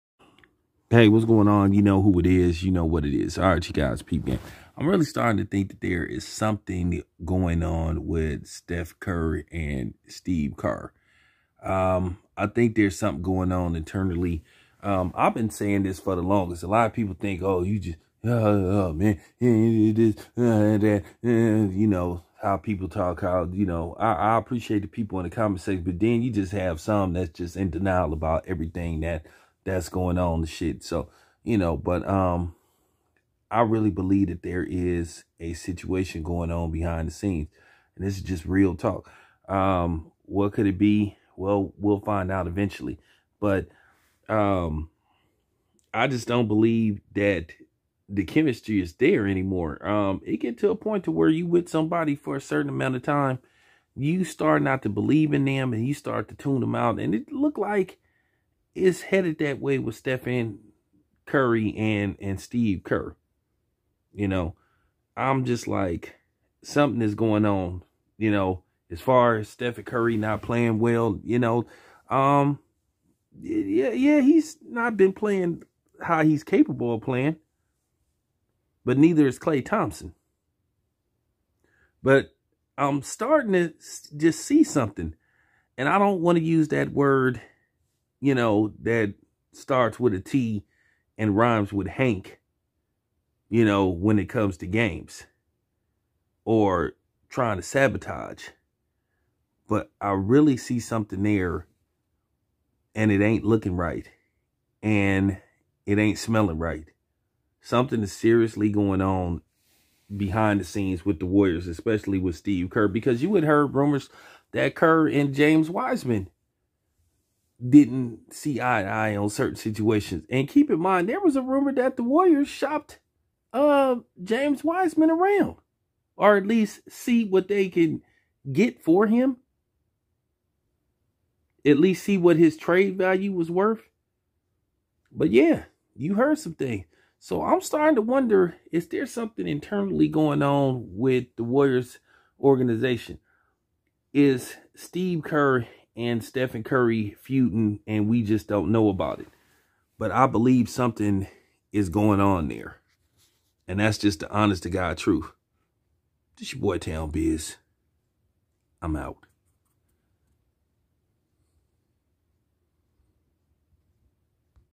something's going on
if there ever was is the chemistry between Steph Curry and Steve Kerr is starting to fizzle out